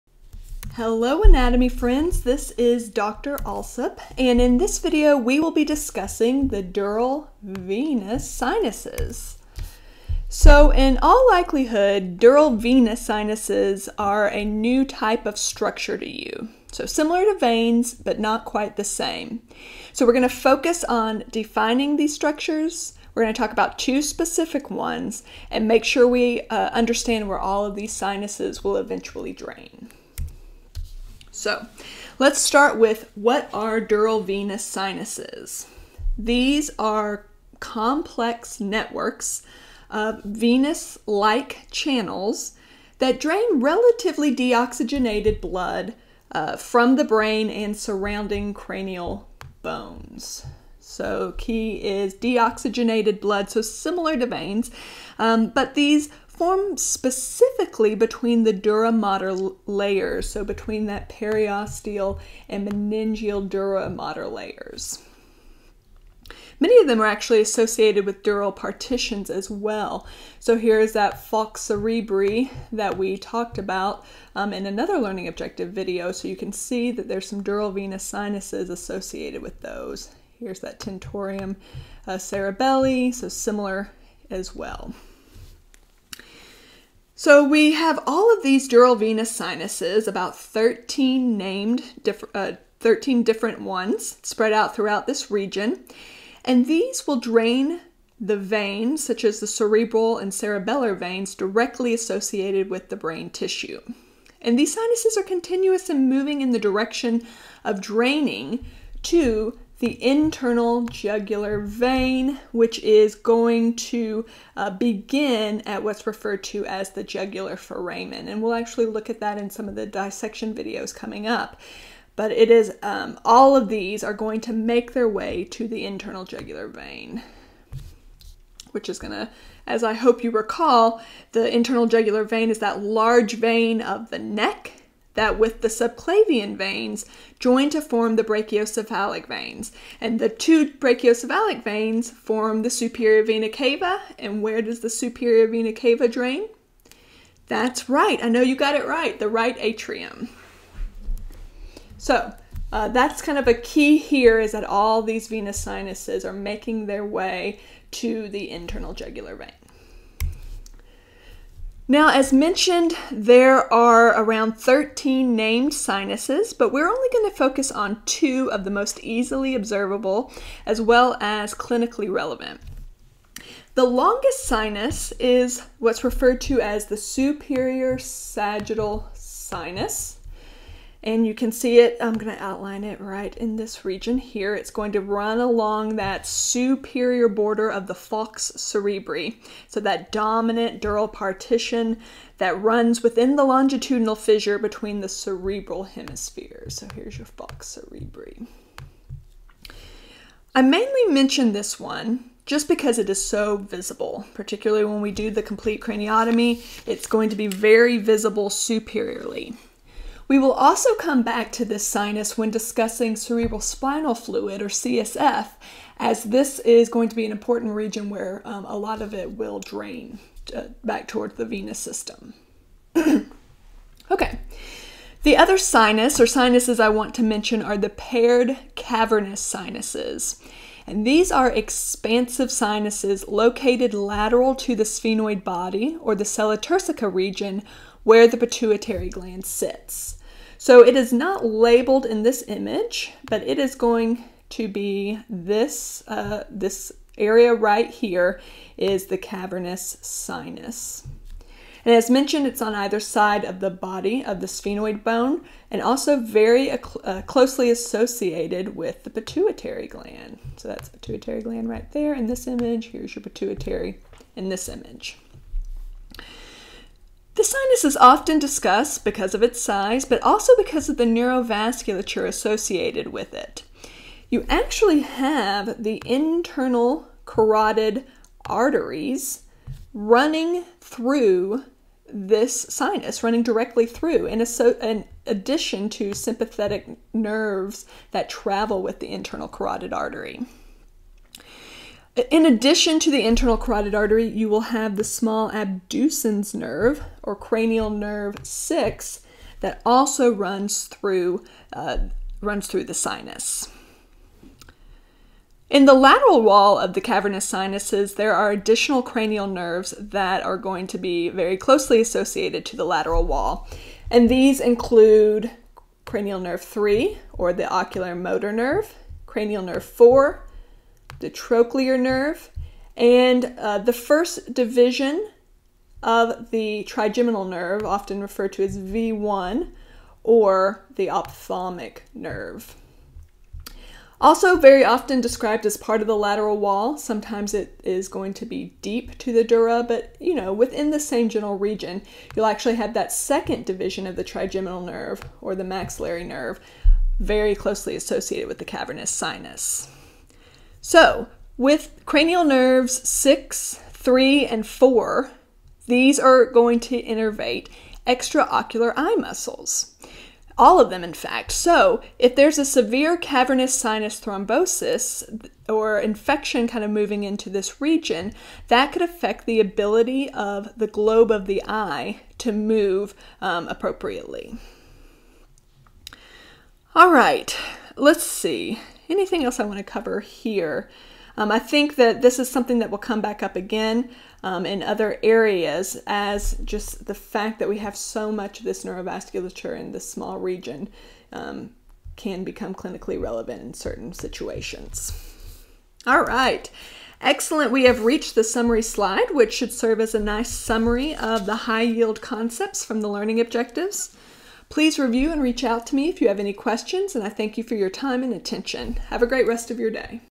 Dural Venous Sinuses - M1 Learning Objectives
Dr. Kathleen Alsup discusses the dural venous sinuses in relation to the learning objectives for the First Year Medical Gross Anatomy course at the University of Michigan Medical School.